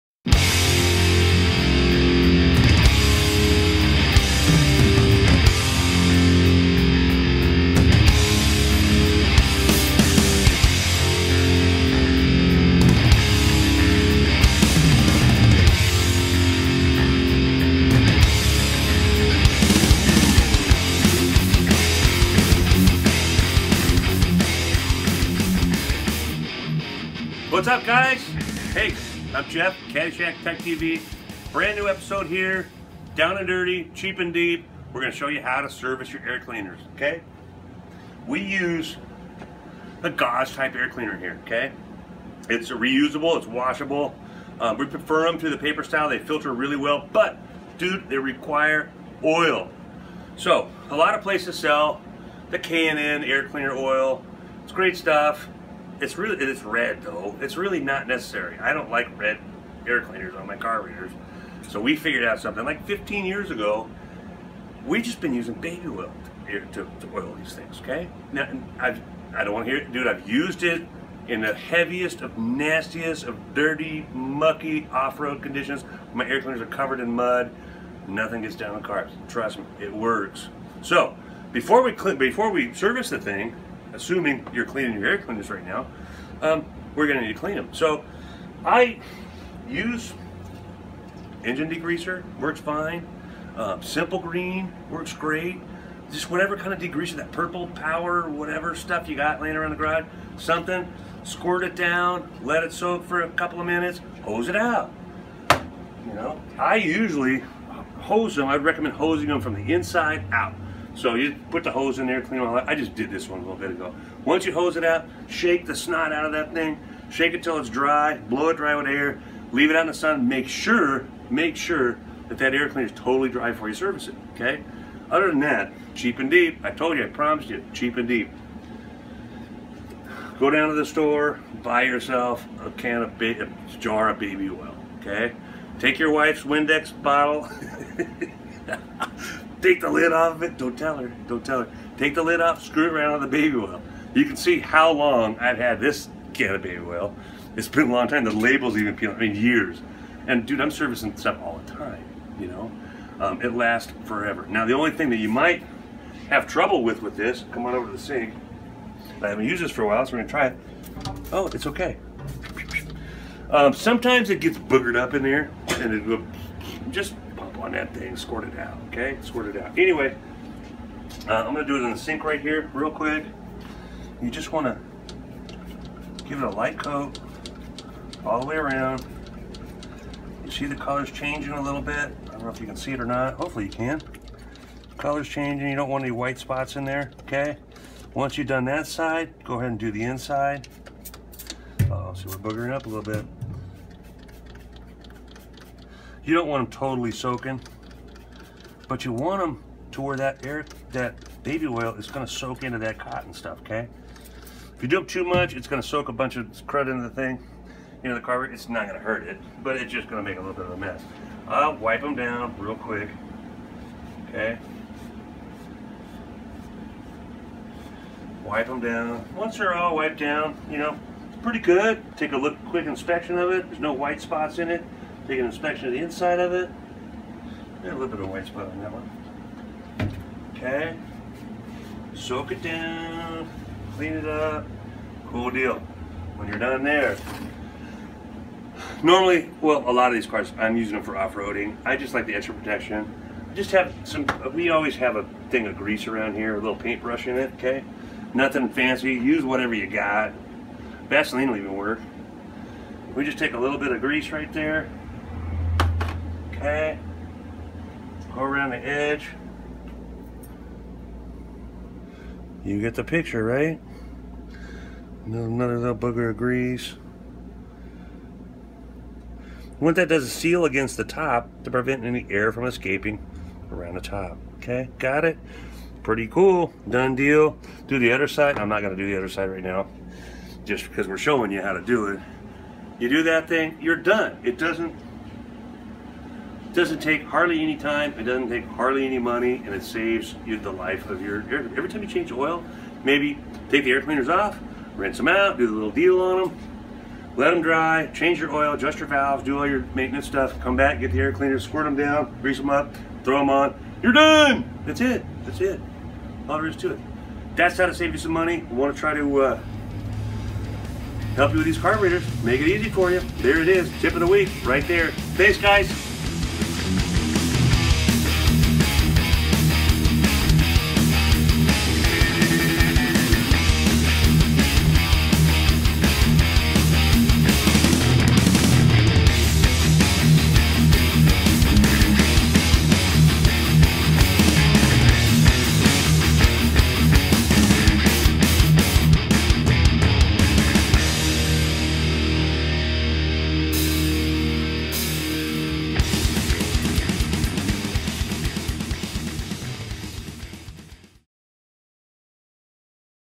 Kaddie Shack Tech TV: How To Service Dual Carb Air Cleaners
Jeff Lain of the Kaddie Shack shows how to service dual carburetor air cleaners for air cooled VW engines.